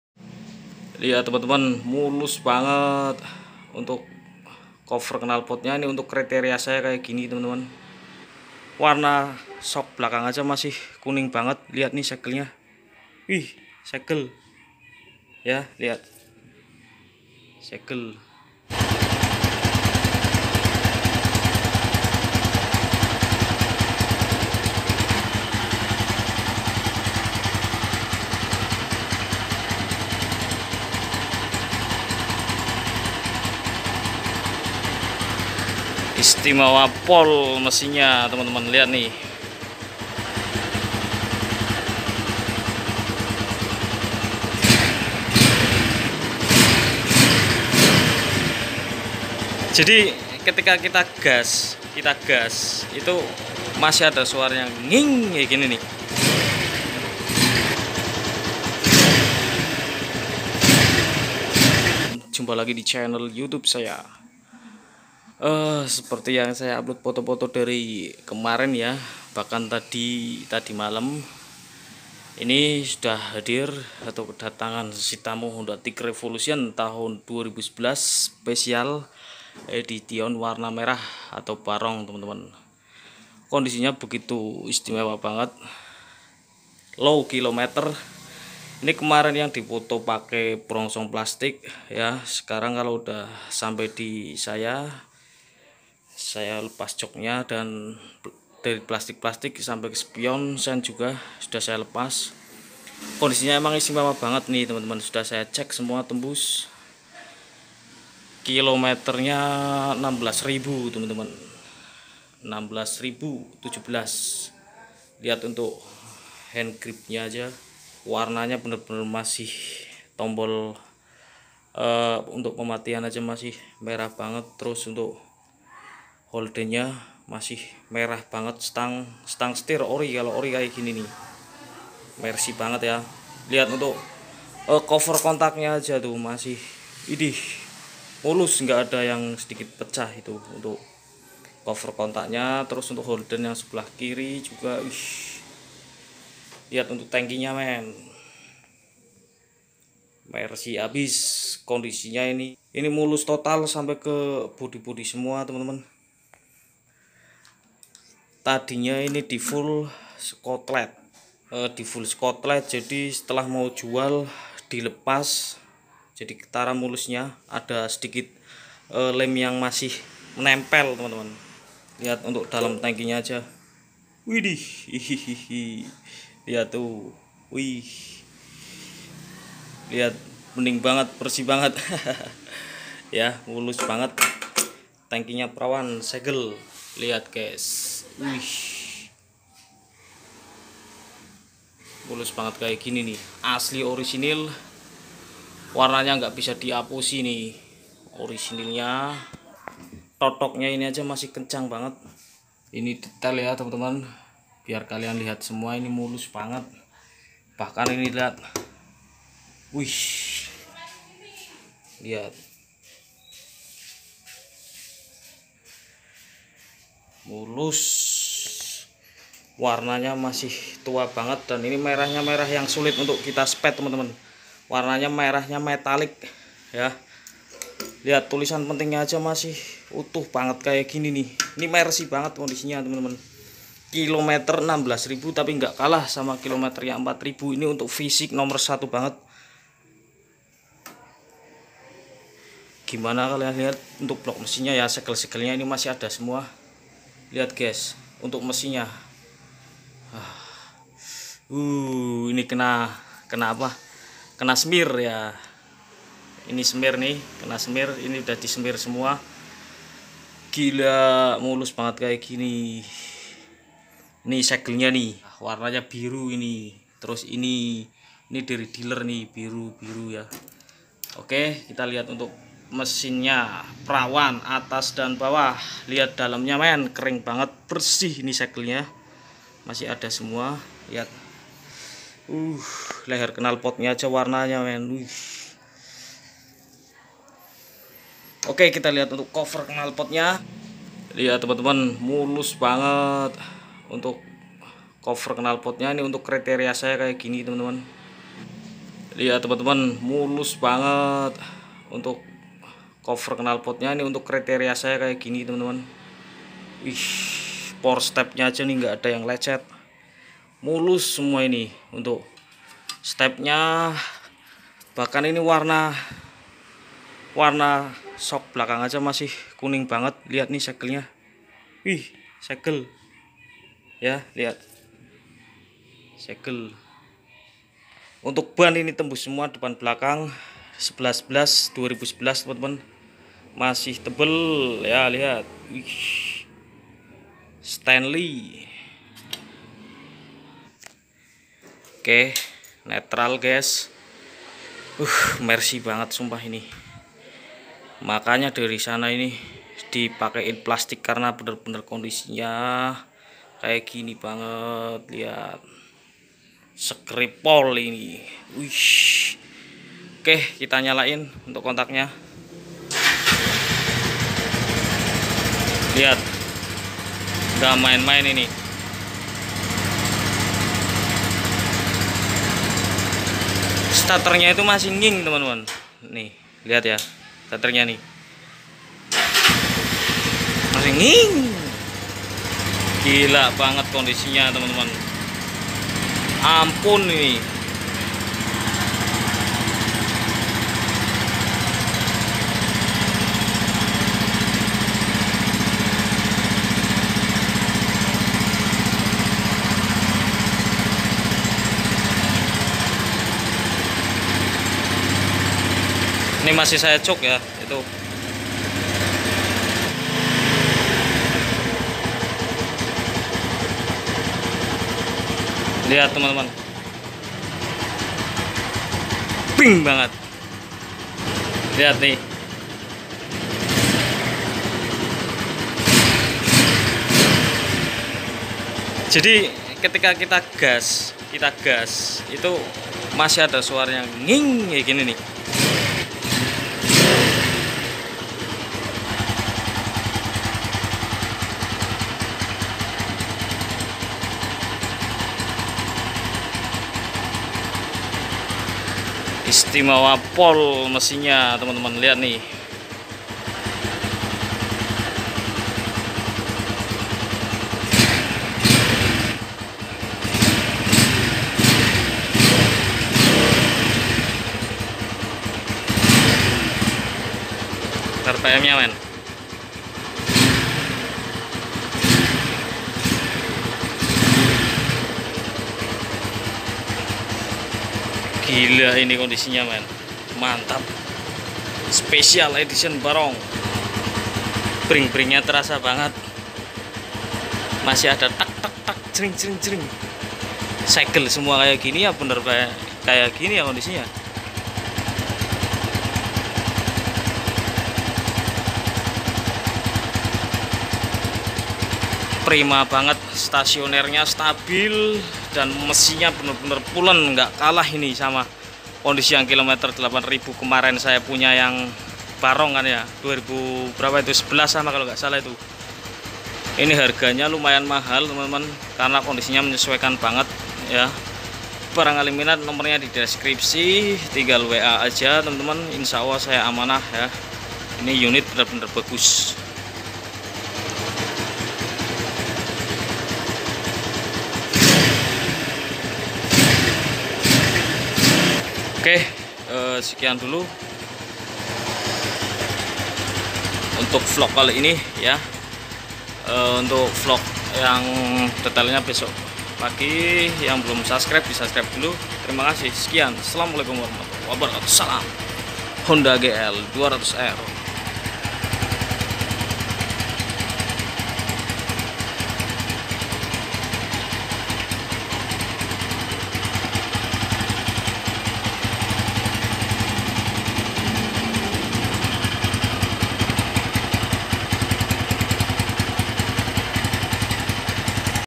Baru masuk TIGER REVO SE MERAH 2011 Kondisinya Super Merzi 100% Ori Segel
Tiger Revo 2011 SE MERAH BARONG Low Kilomter 16 ribu... Sangat istimewa Full bodi mulus total ORIGINAL